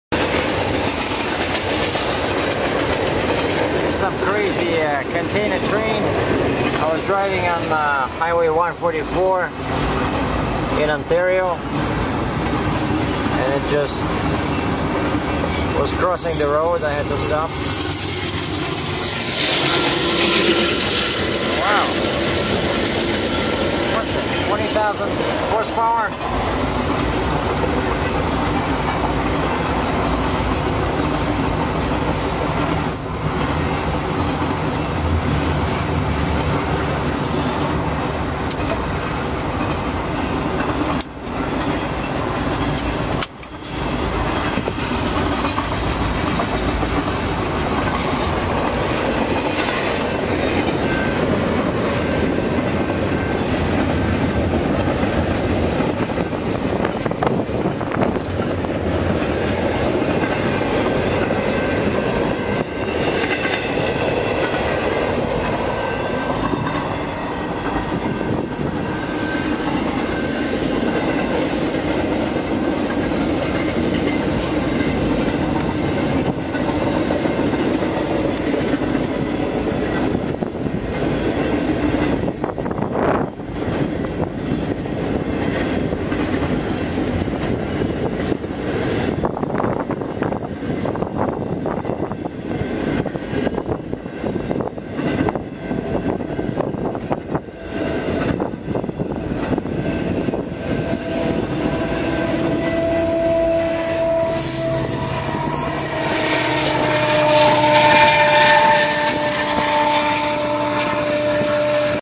CONTAINER TRAIN FLIES ACROSS HWY 144 IN CANADA
I was driving north on Hwy 144 in ON, Canada near the town of Gogama when I saw a massive container train up ahead crossing my way on some kind of a bridge.. As I came closer I realized there was no bridge and the train was indeed rolling across the highway! I had less than 1/2 of a mile to slow down from 57 MPH to 0 ... I wish they had a barrier at this crazy railroad crossing so that motorists can see it earlier and spare their brakes!